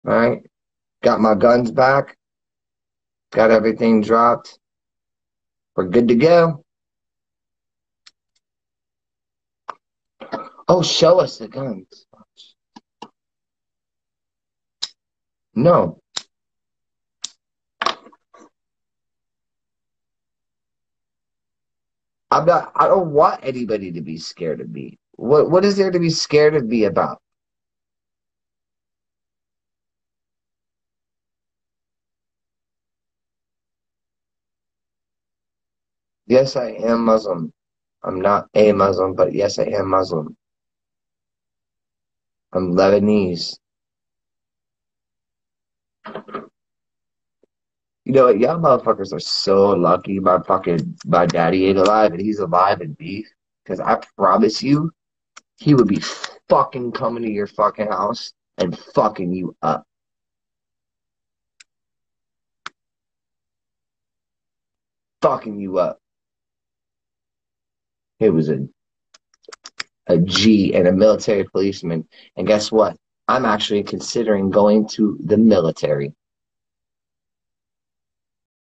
Aaron Carter - Is joining the Military. Also shares his “Religion, got guns back & suits dropped”
Aaron Carter - “Team Lulu Here”!  Who knew! He’s joining the military! Also shares that he is now Muslim along with he got his guns back, and all “charges” have been dropped.